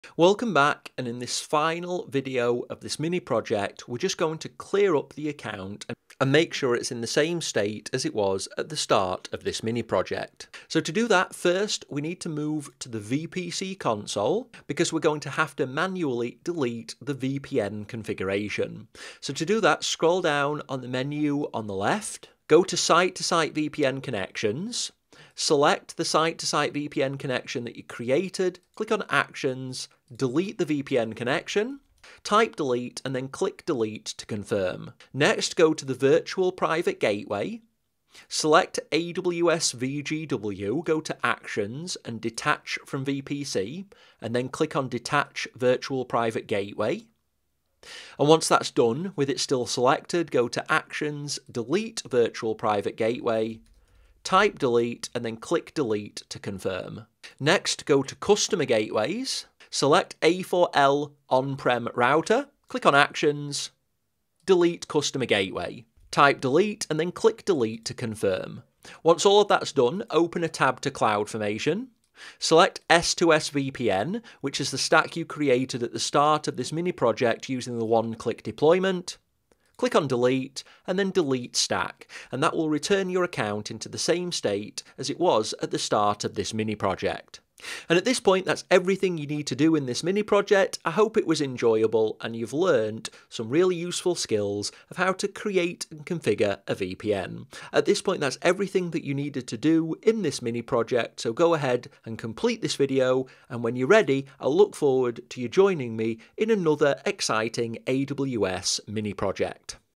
Mini Project - Simple Site2Site VPN STAGE5 - CLEANUP
In this mini project you will implement a site to site VPN between AWS and a simulated on-premises business site running the pfSense router/NAT software.